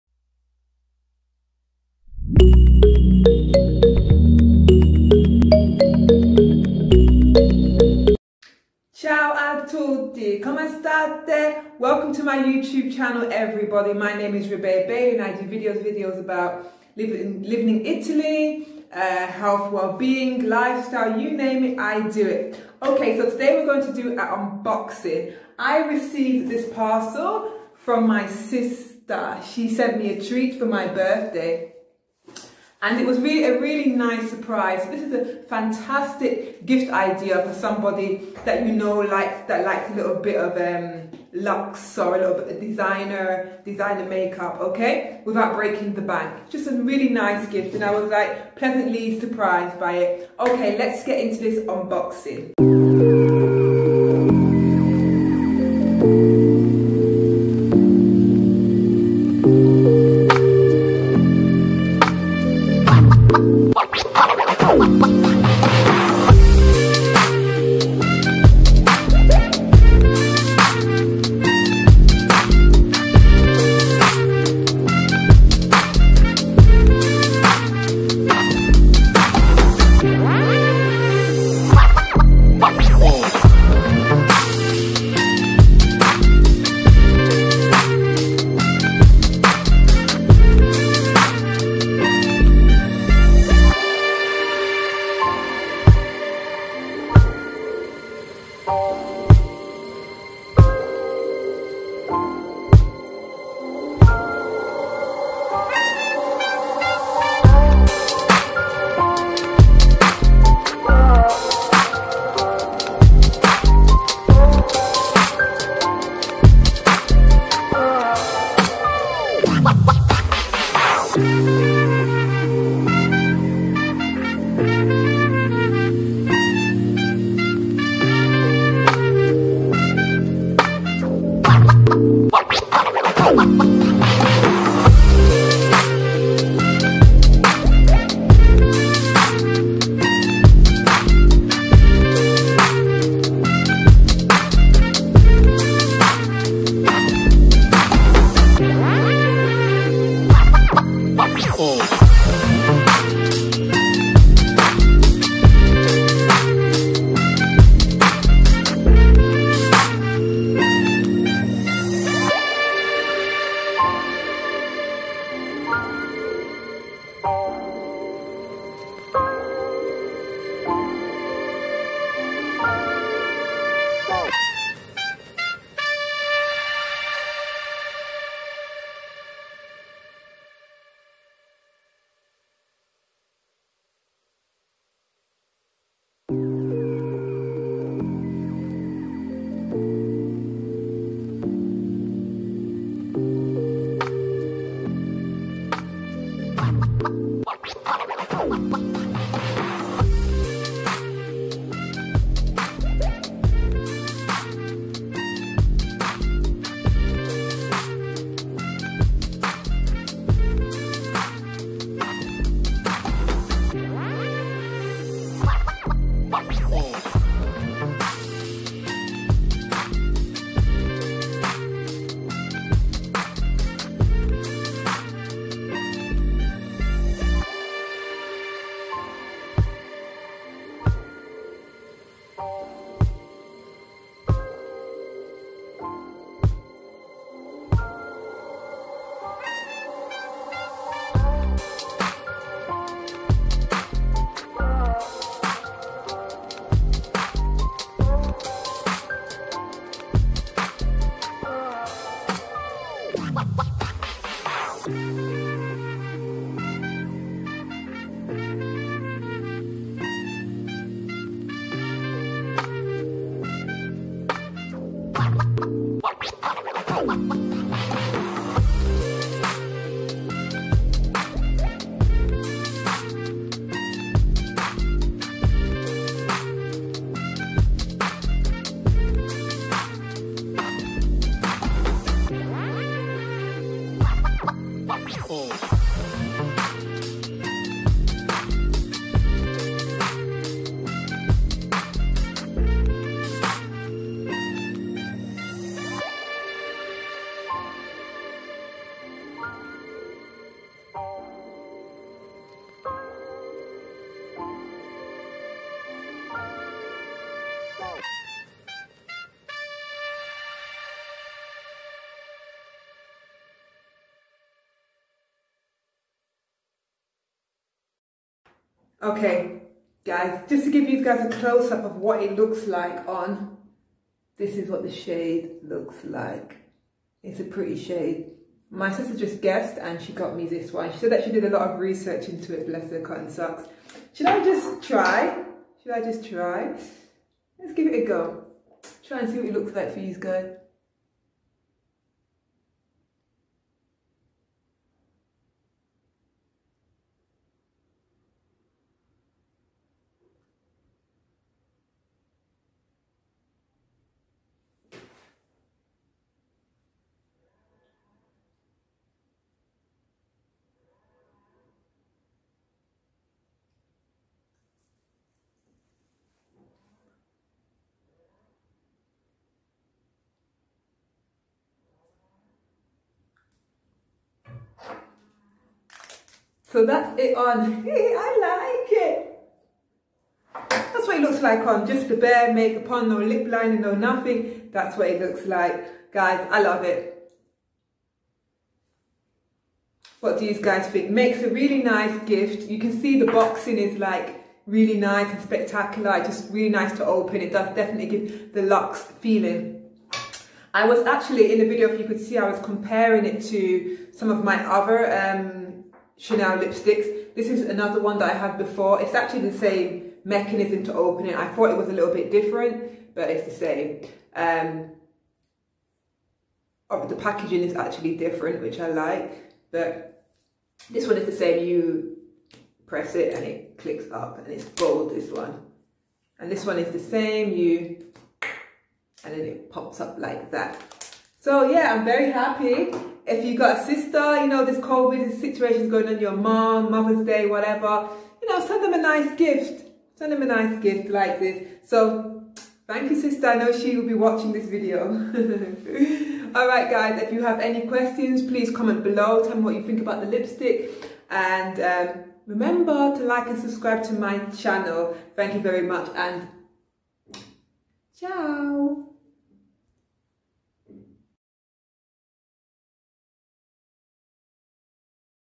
UNBOXING CHANEL LIPSTICK -ROUGE ALLURE VELVET 116 VELVET EXTREME-
Hey Beauties,
In this video, I will be unboxing ROUGE ALLURE VELVET 116 VELVET EXTREME lipstick. I will give you a few tips.

Sending love and light xoxo